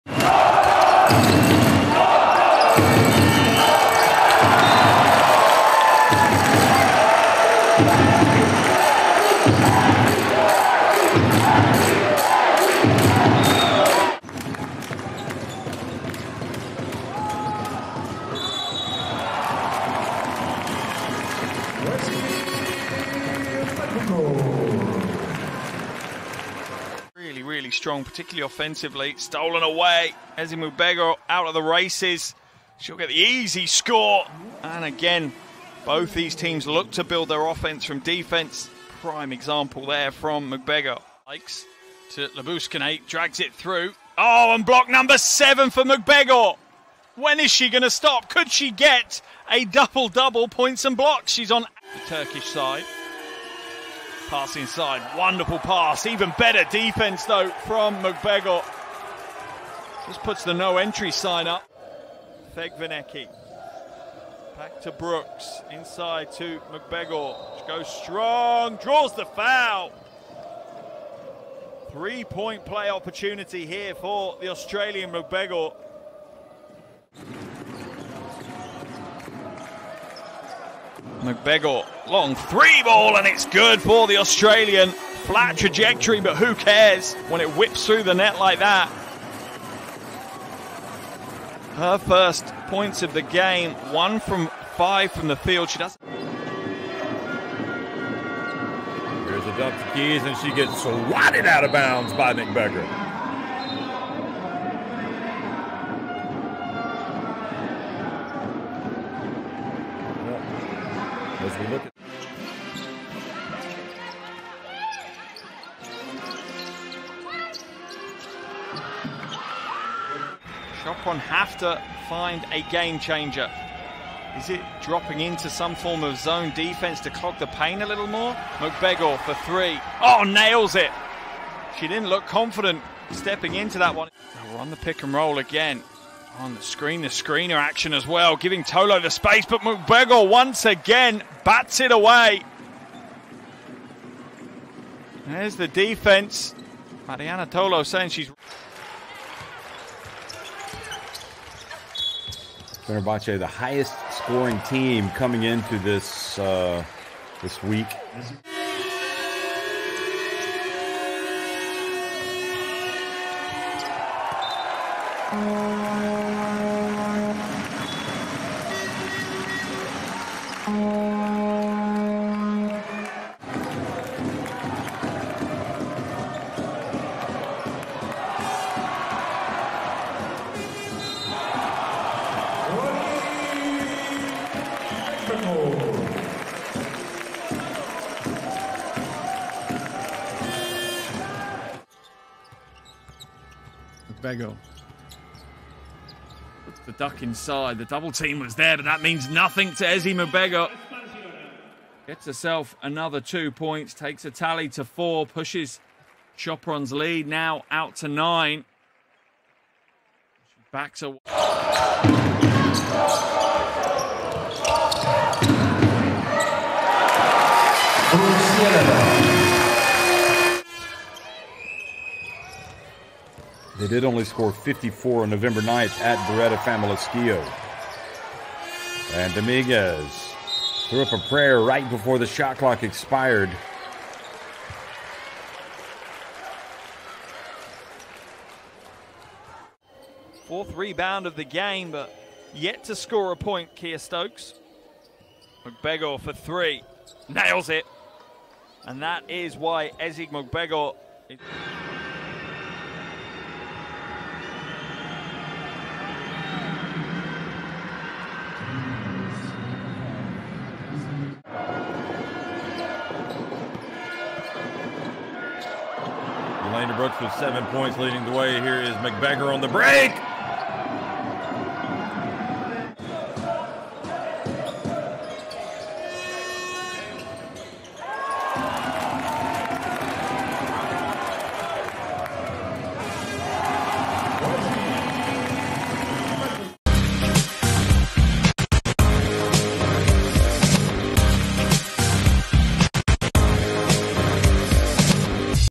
Ezi Magbegor | Sopron Basket | EuroLeague Women 2022-23 Season Full Highlights
Ezi Magbegor is a star player for Sopron Basket, one of the top teams in the EuroLeague Women 2022-23 season. She is a versatile and athletic forward who can score, rebound, and defend at a high level. In this video, you will see some of her best highlights from the season, including her amazing dunks, blocks, and passes. Watch how she dominates the competition and leads her team to victory. Ezi Magbegor is a force to be reckoned with in the EuroLeague Women!